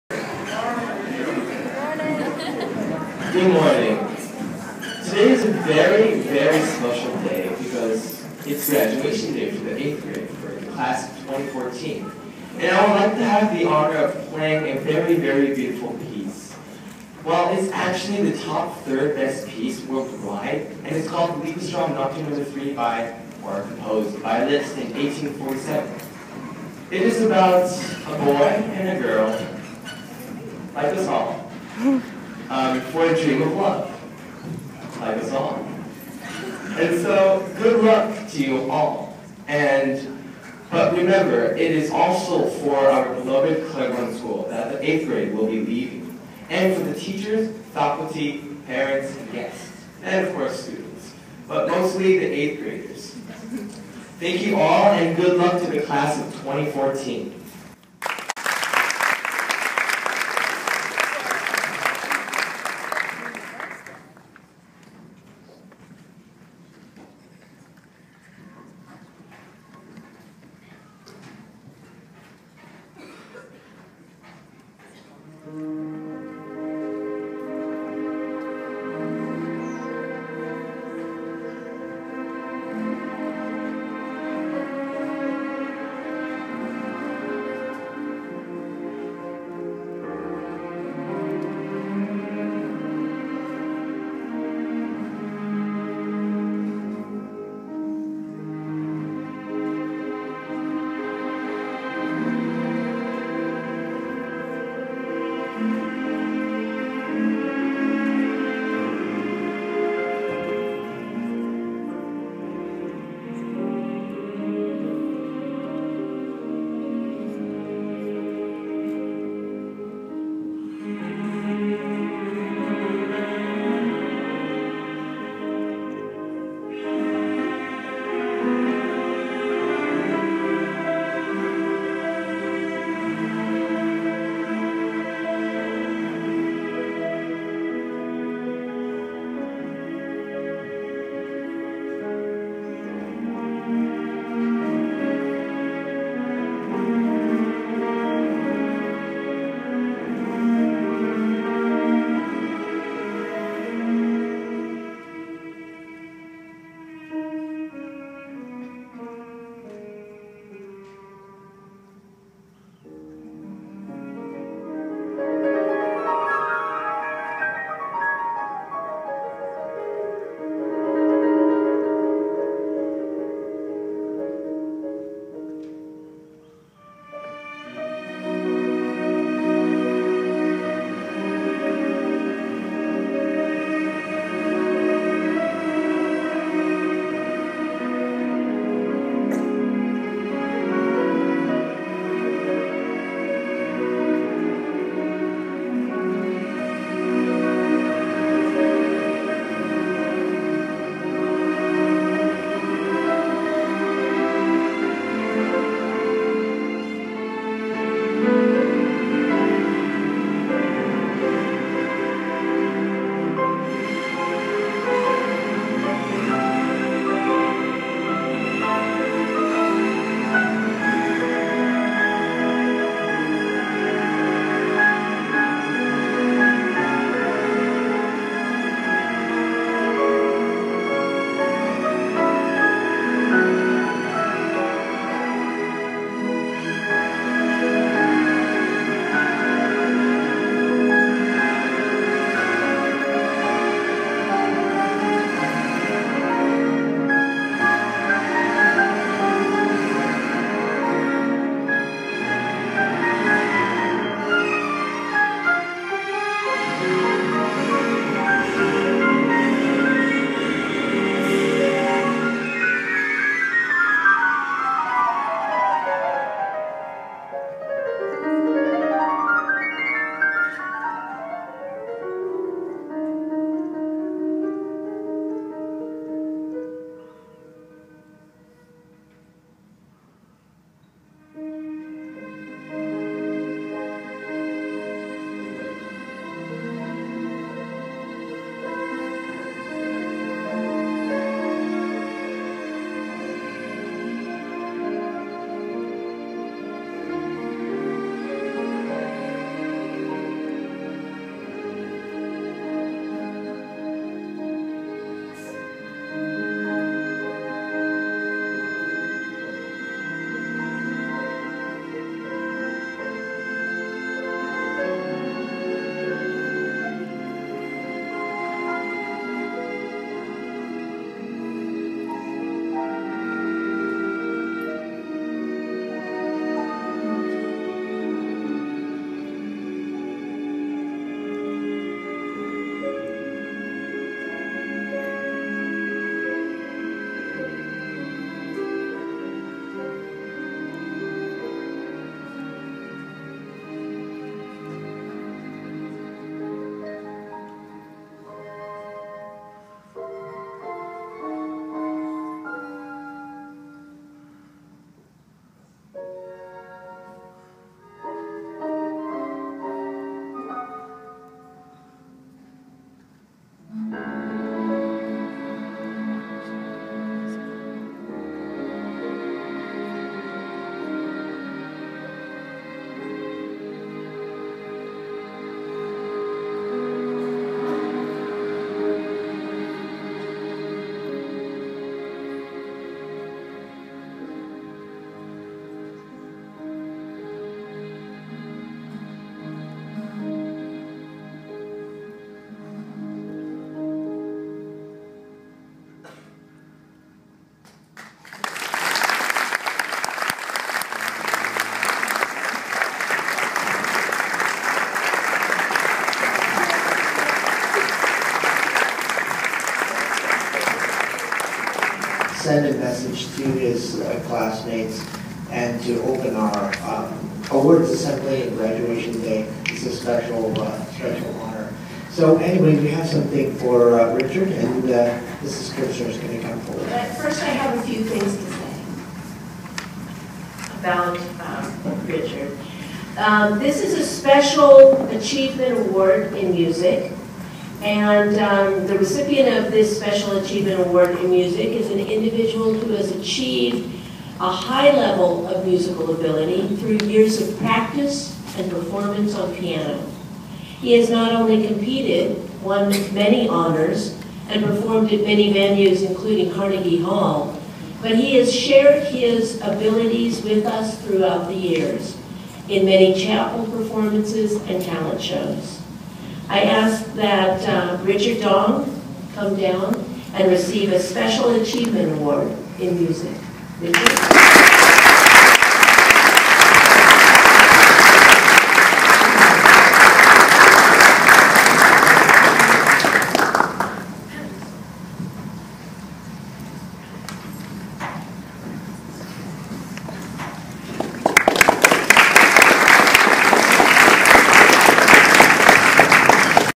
Clairbourn Alumnus Richard Dong Plays Piano on Graduation Day at Clairbourn in 2014
At age 13, Clairbourn Alumnus Richard Dong gave a moving paino performance on graduation day and receives the Clairbourn School Special Music Award 2014.

    June 2014